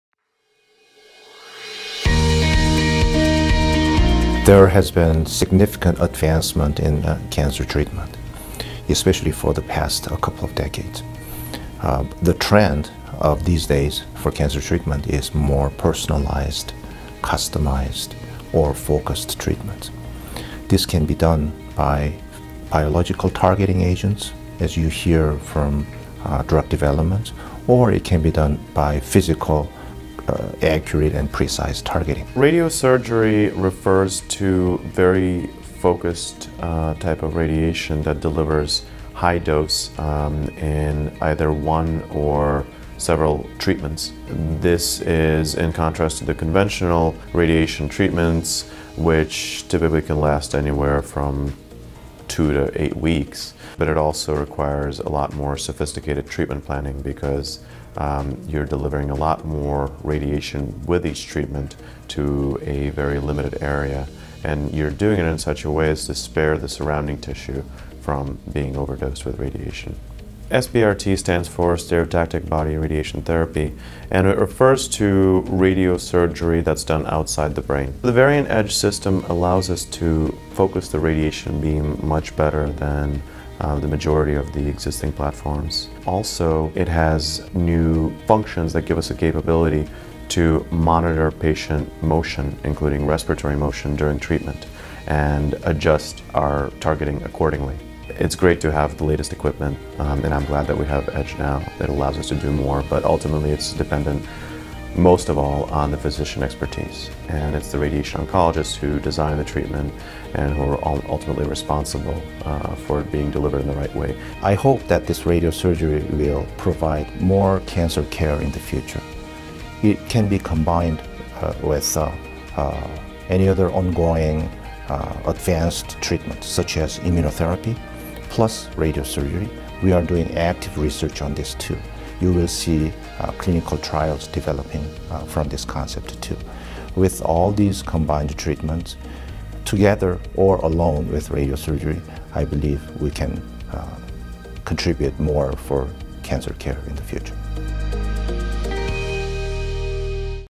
Pushing Radiosurgery to the Edge™
The advanced Varian® Edge™ radiosurgery system offers a new line of defense in the fight against cancer at Stony Brook University Cancer Center. In this non-invasive procedure, the radiation oncologist delivers highly targeted treatment to the tumor, while sparing the healthy area around it.